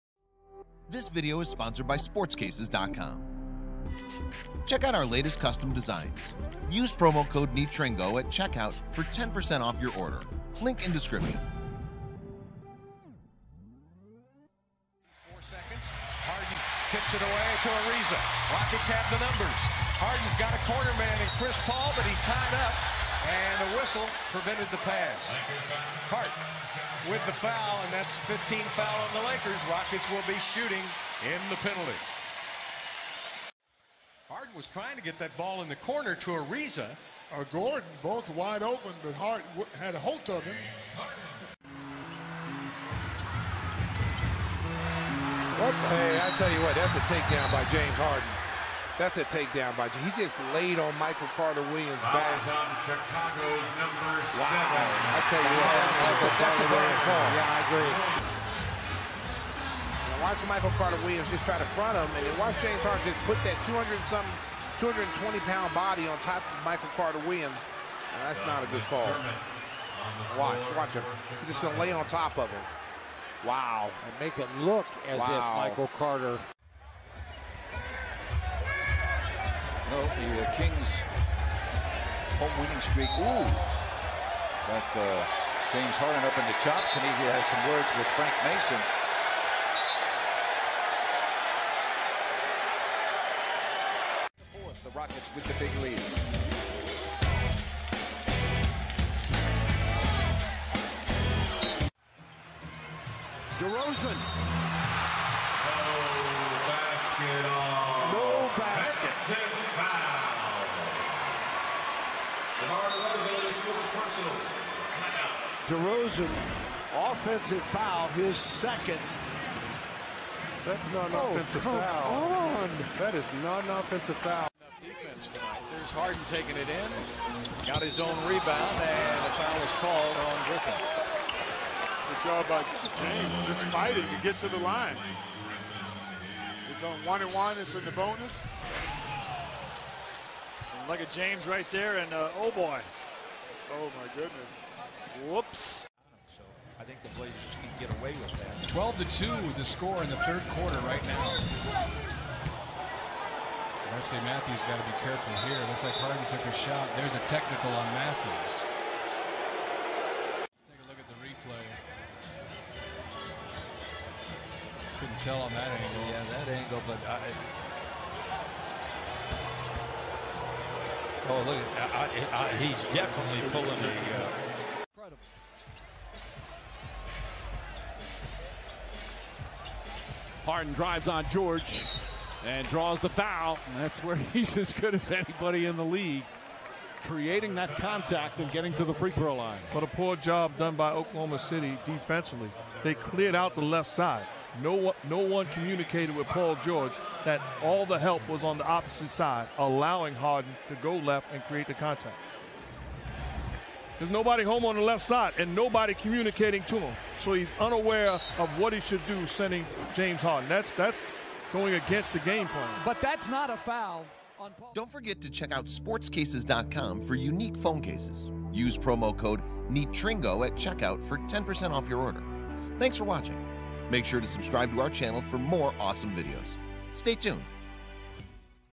"FUNNIEST FOULS" Created By James Harden
James Harden is the GOD of drawing Contacts/Fouls!

Source: NBA!